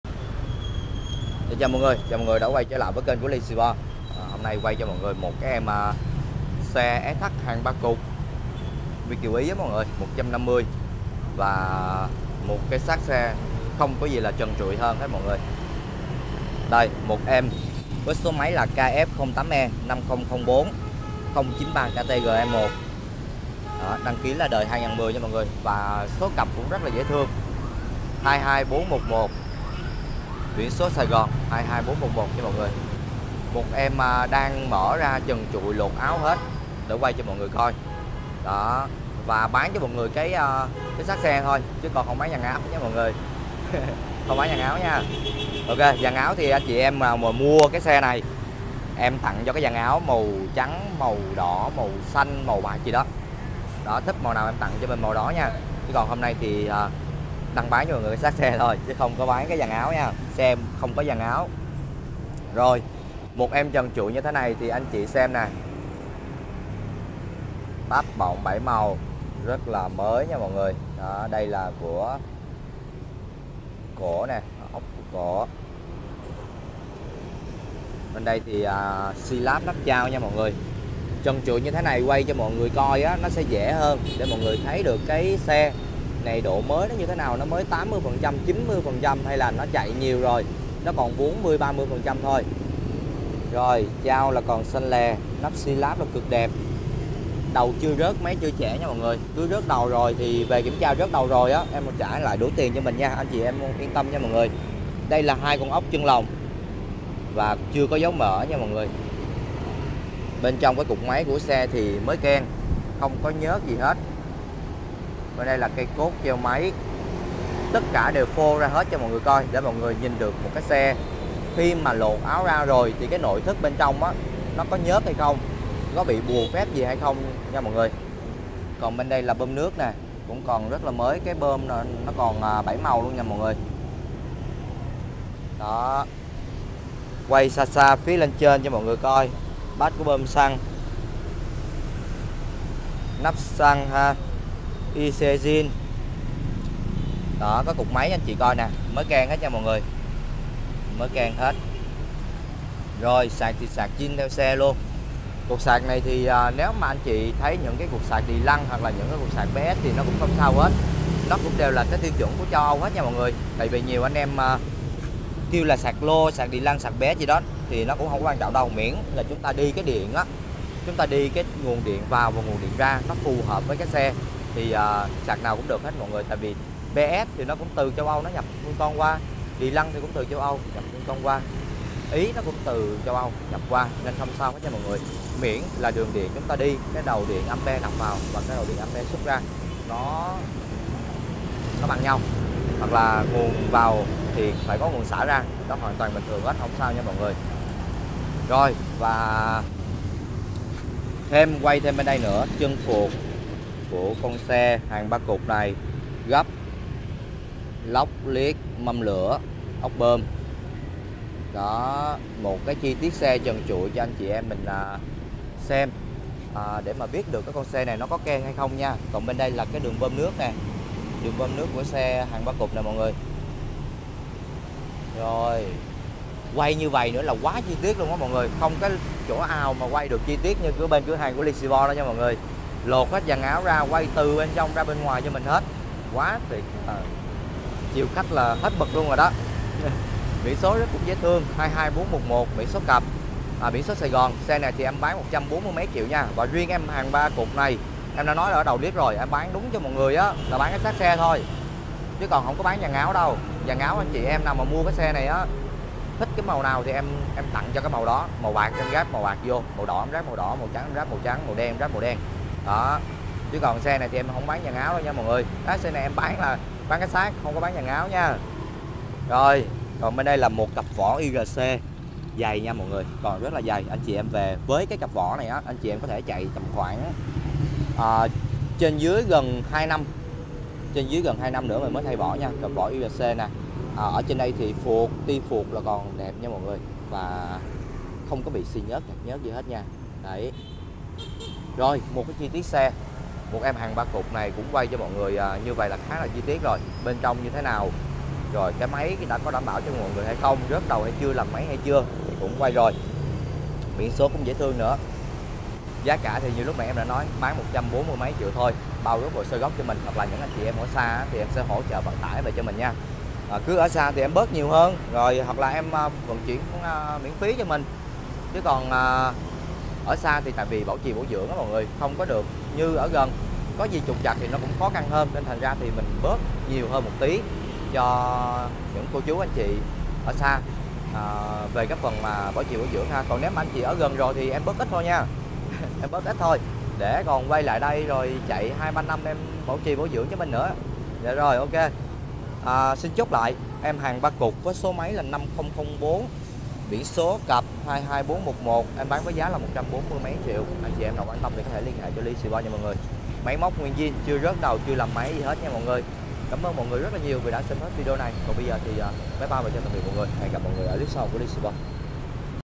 Mở Áo SH Ý Quay Chi Tiết Cục Máy Nguyên Zin Hàng Việt Kiều Ý | Lysport
👉 Ly Sport , 829 Lũy Bán Bích, Phường Tân Thành, Quận Tân Phú, HCM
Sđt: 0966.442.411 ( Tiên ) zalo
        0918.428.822 ( Hoàng xì po) zalo